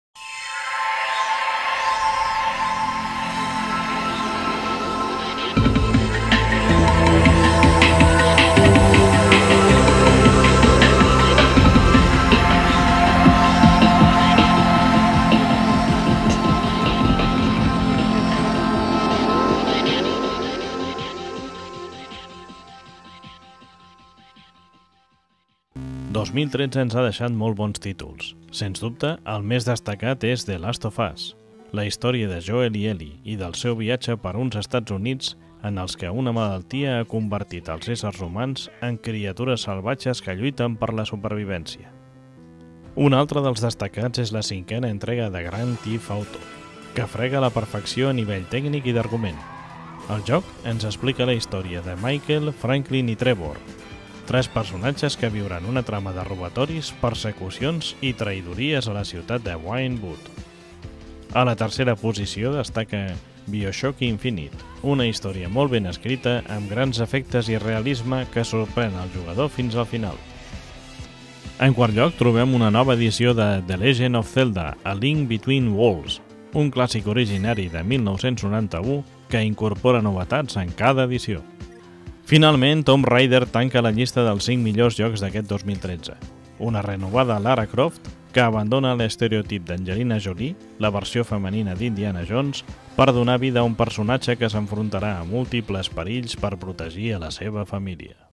Els millors videojocs del 2013
Aquest any que ara s’acaba, ens ha deixat molt bons títols. Se’ns dubte el més destacat és ‘The last of us’, la història de Joel i Ellie, i del seu viatge per uns Estats Units en els que una malaltia a convertit als essers humans en criatures salvatges que lluiten per la supervivència.

Un altre dels destacats és la cinquena entrega de ‘Grand theft auto V’, que frega la perfecció tècnicament i en el seu argument. El joc ens explica la història de Michael, Franklin i Trevor, tres personatges que viuran una trama de robatoris, persecucions i traïdories a la ciutat de Vinewood.

A la tercera posició destaca ‘BioShok Infinite’, una història molt ben escrita, amb grans efectes i realisme que sorprèn al jugador fins al final.

En quart lloc troben una nova edició de ‘The legend of Zelda: A link between worlds’, un clàssic originari de 1991 que incorpora novetats en cada edició.

Finalment, ‘Tomb Raider’ tanca la llista dels 5 millor jocs d’aquest 2013, una renovada Lara Croft que abandona l’estereotip d’Angelina Jolie, la versió femenina d’Indiana Jones, per donar vida a un personatge que s’enfrontarà a múltiples perills per a protegir la seva família.